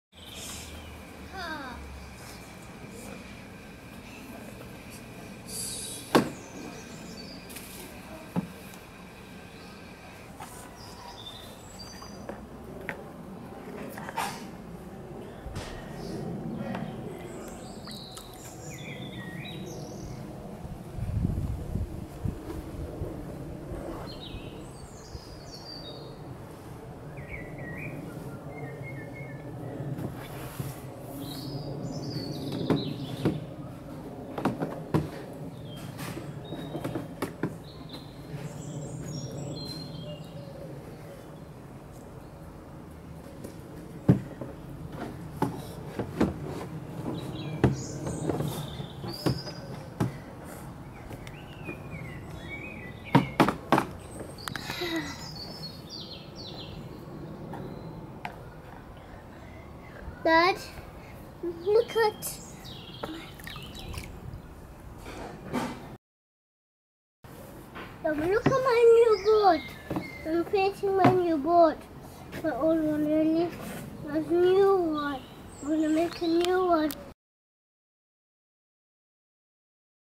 Wood Pigeon Pecking Away in a Cherry Tree
Found a young wood pigeon high up in a cherry tree pecking away in a tree that just finished blossoming.